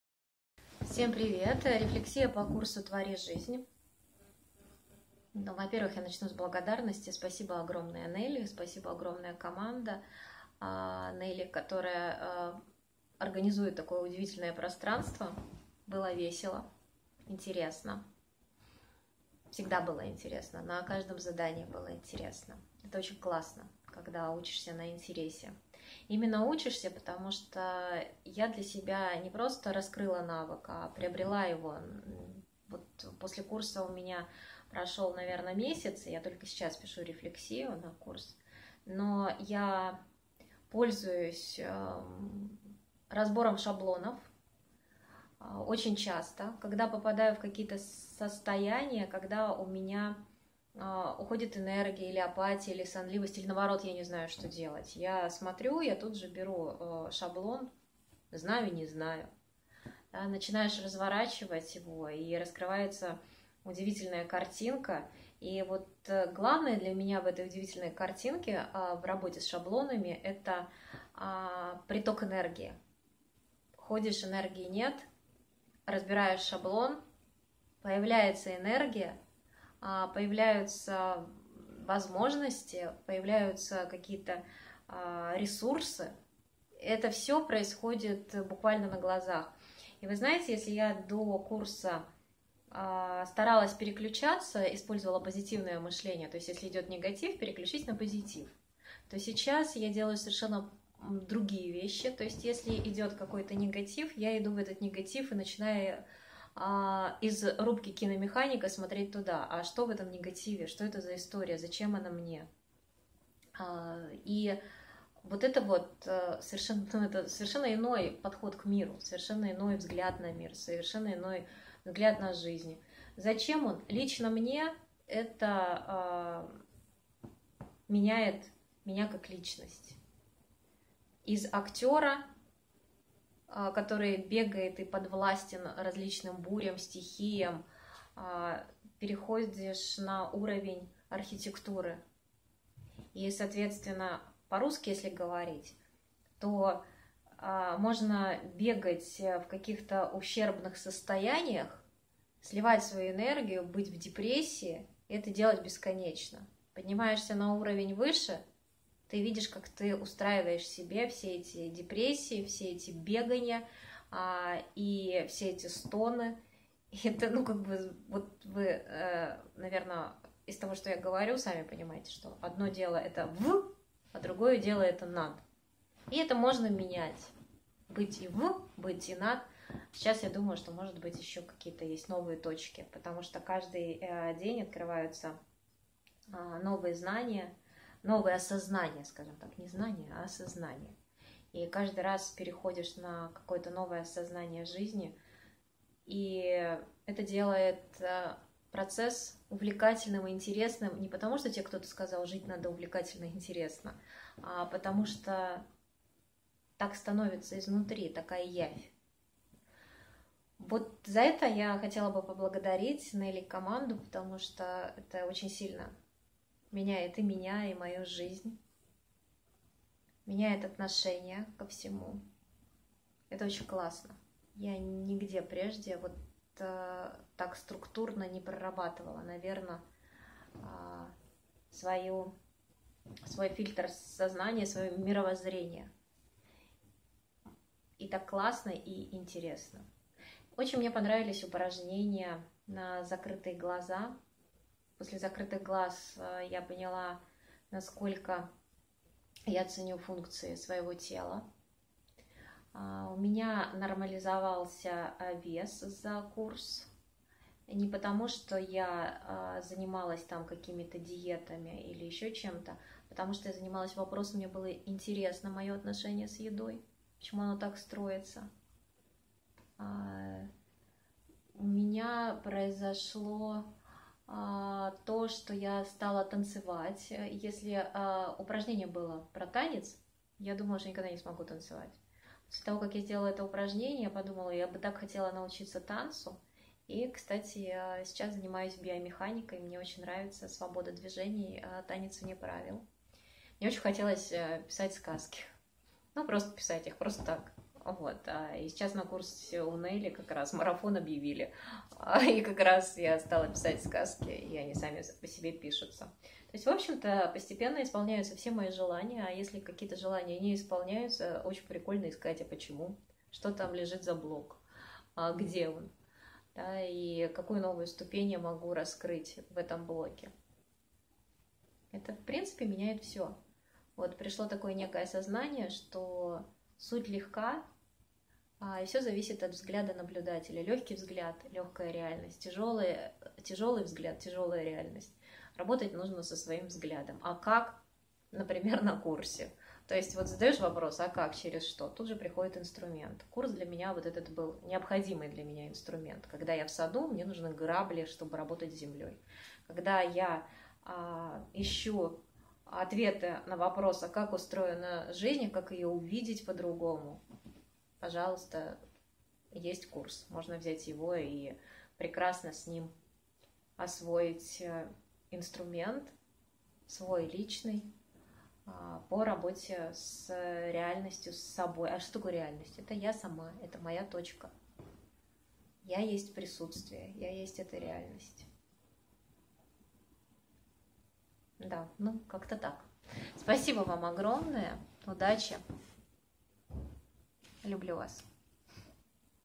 ОТЗЫВ О КУРСЕ «ТВОРИ ЖИЗНЬ» НЕЛЛИ ДАВЫДОВОЙ ❤️ (2021)
Если кратко: будет работа с шаблонами мышления, с противоречивыми желаниями, откуда корень ваших проблем, как его распознавать, как эту проблему решить, как исполнить желание, как проработать не только свои проблемы, но и близких!
Немного волшебства, много чувств, радости и любви. Открытие себя настоящего, ресурсов, талантов. И, конечно же, любви к себе!